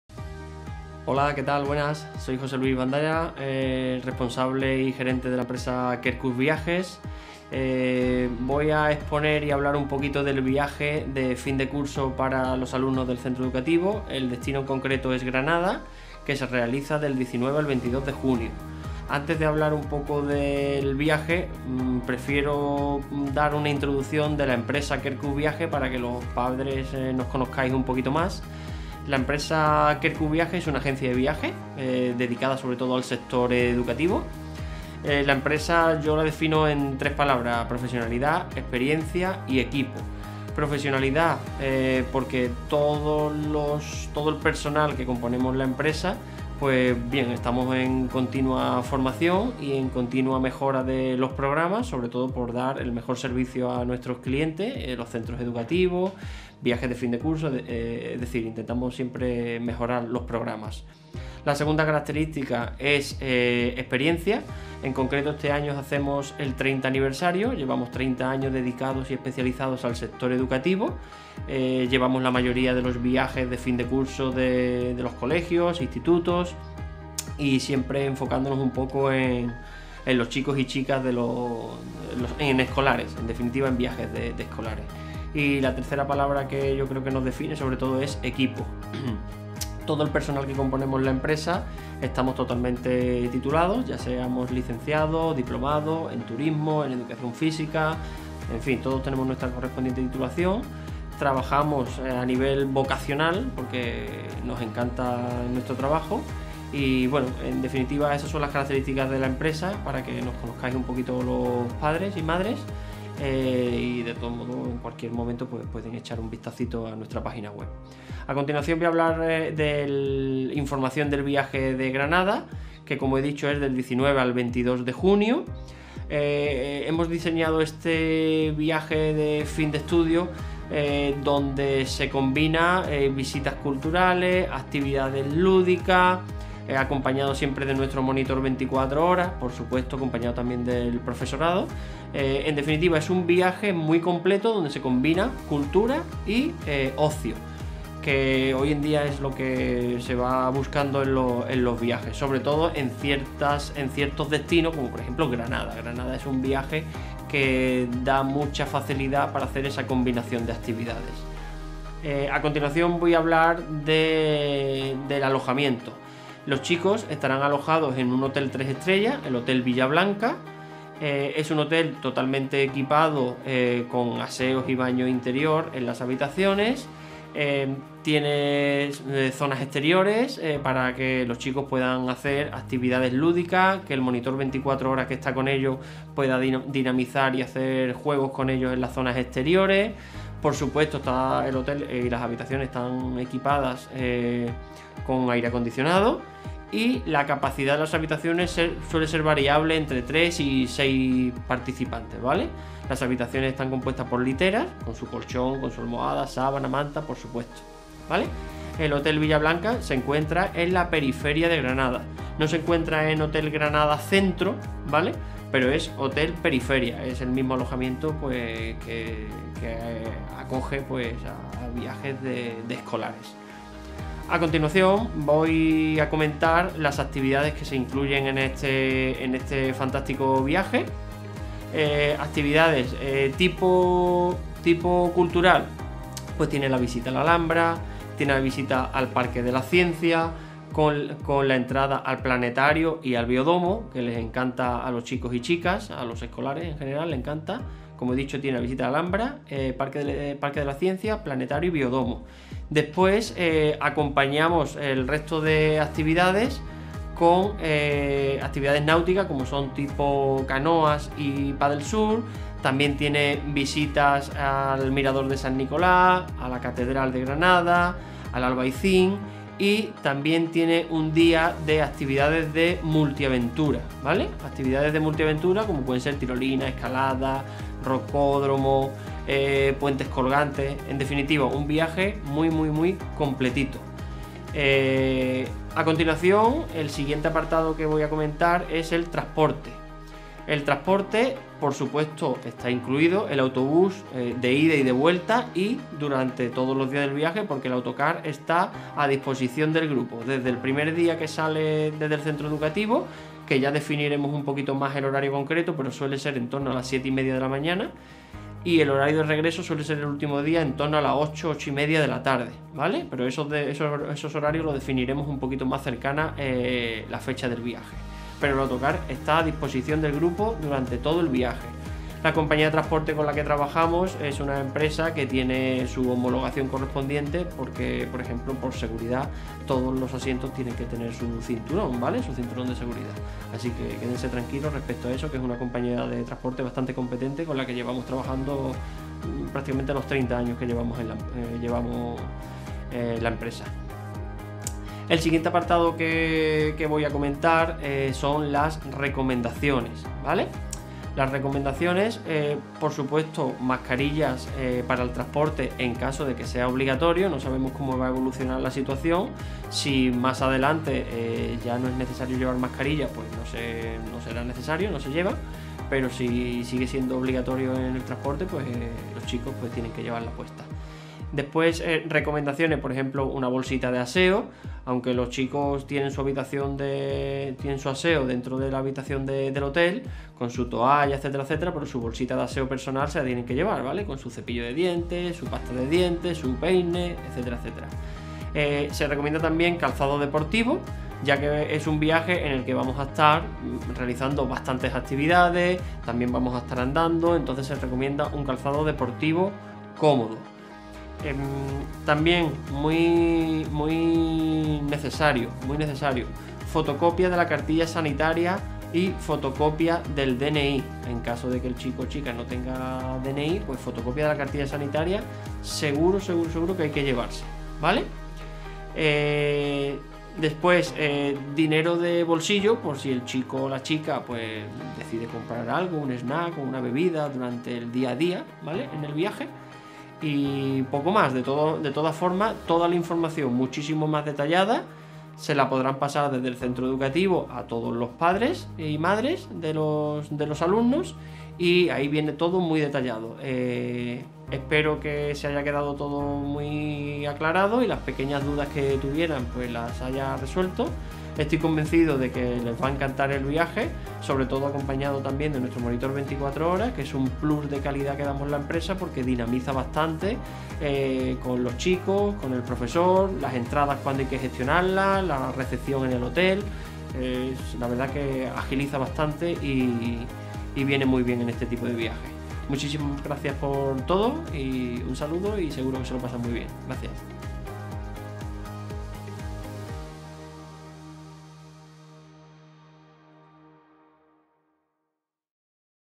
Information Video - Sunny View School Trip to Granada (Year 8)
Information by the Querkus Agency, which will be in charge of our School trip to Granada taking place from 19-22 June, 2023.